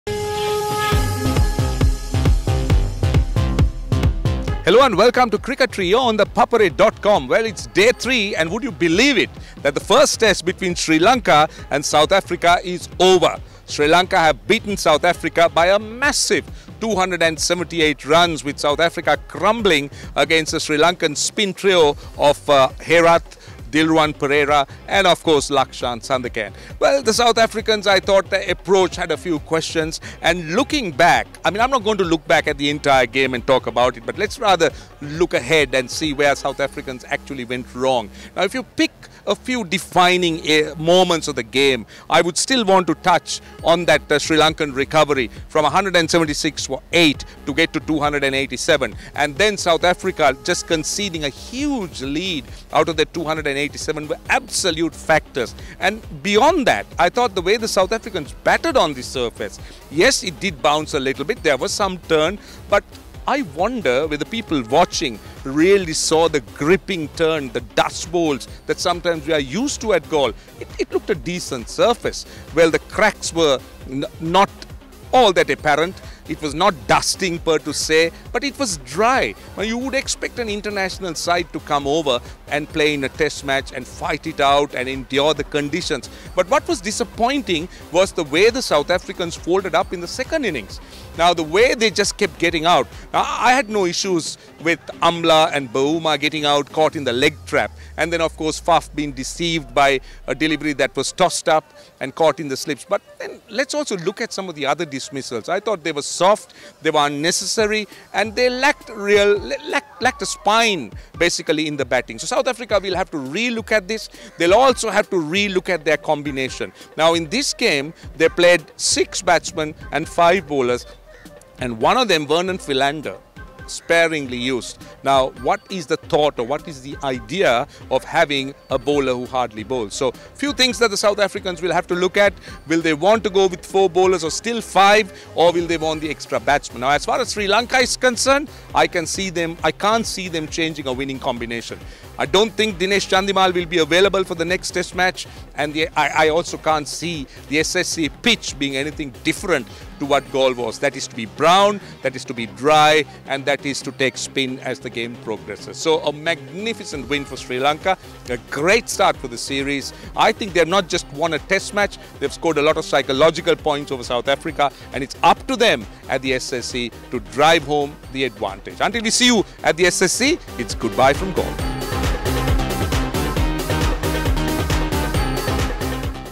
South Africa showed no fight in Galle – Day 3: Cricketry
Renowned Cricket Commentator Roshan Abeysinghe analyzing the third day’s play in the opening Test match of the three-match series between Sri Lanka and South Africa in Galle.